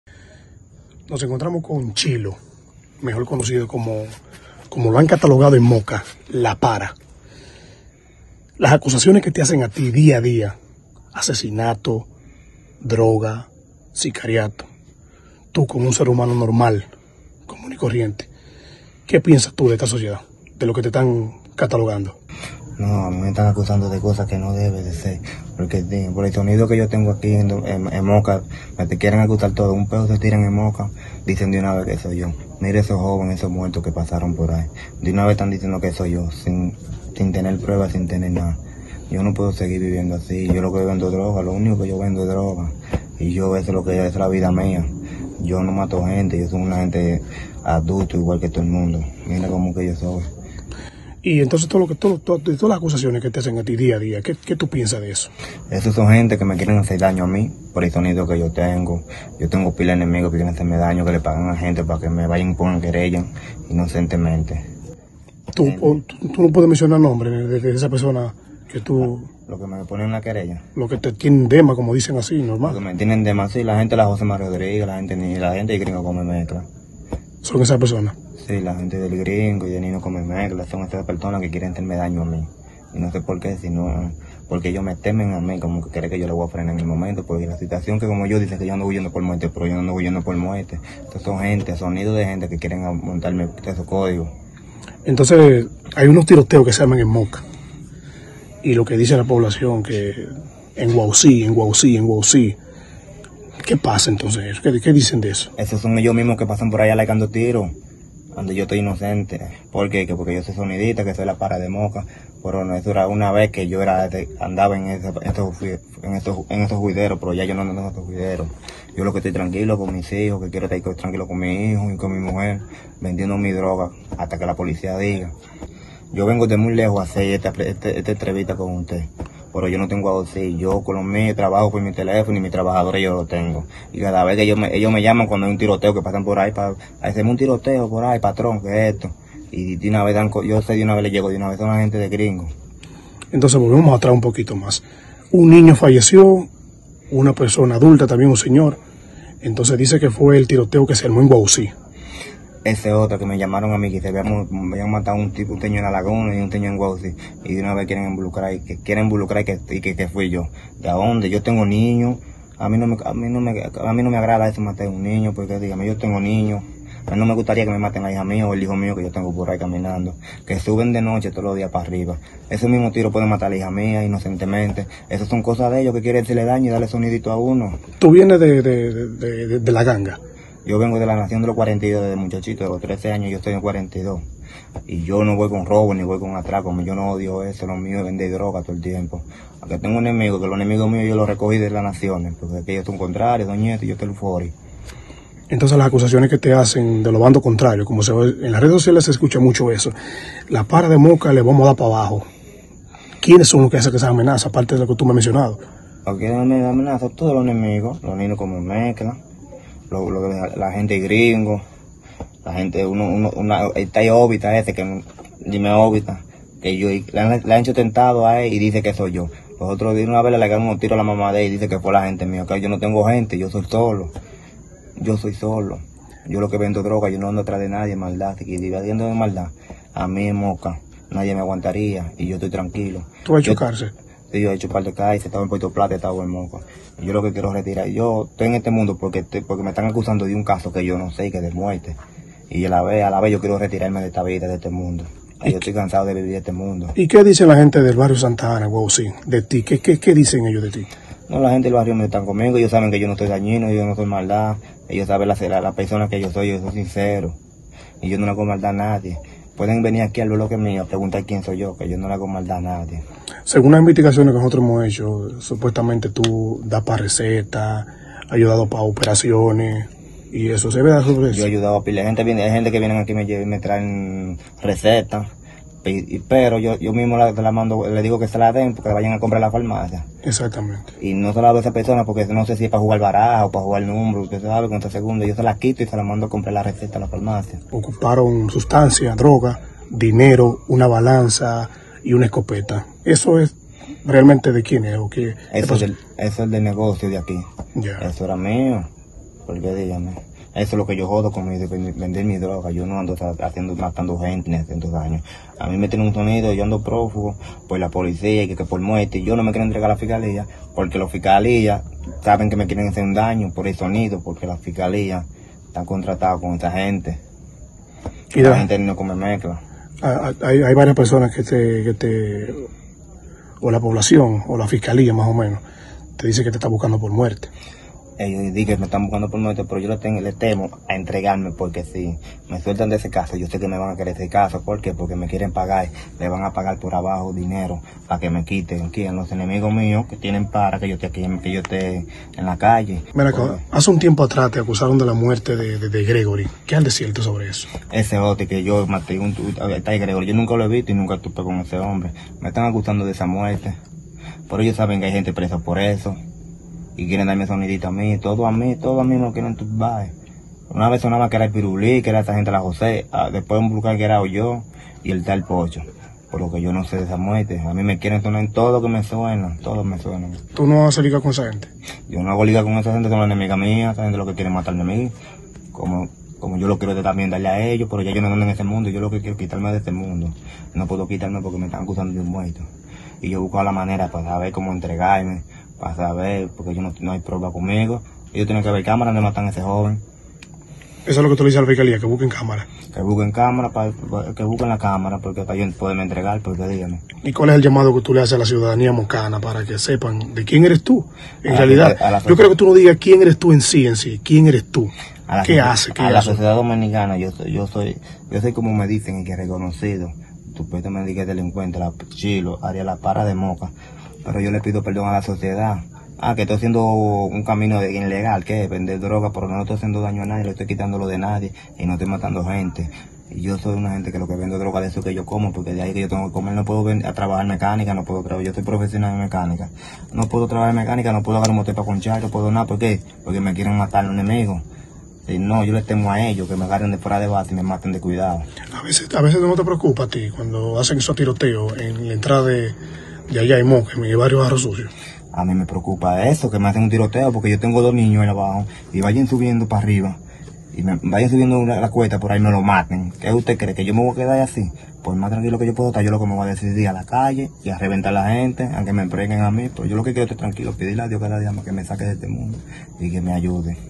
EXCLUSIVA! - CHILO LA PARA EL CAPO MAS BUSCADO DE MOCA
Yonathan Martínez (Chilo) dueño de punto en Moca, dice está vendiendo su droga hasta que la policía diga. habla en Exclusiva con @YeidyBatista y se desliga de las muertes ocurridas en las últimas semanas en Moca.